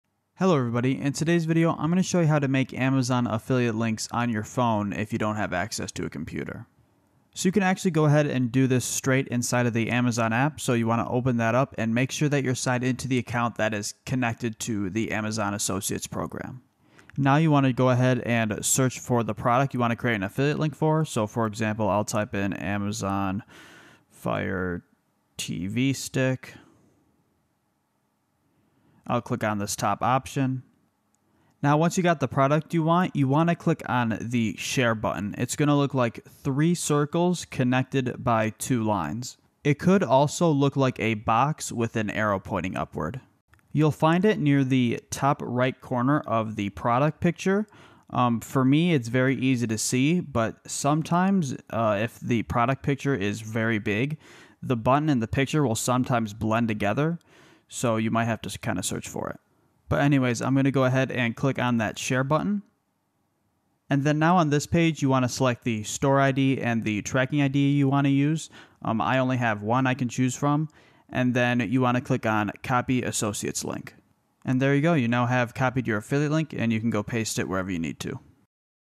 How to Get Amazon Affiliate Links on Mobile - 2025
In this video, I show you how to generate and copy Amazon affiliate links on the Amazon app! Creating Amazon affiliate links is easy to do on your Amazon Associates Account.

0:00 - Intro
0:07 - How to get Amazon Affiliate Links on Mobile

-SWIFTNESS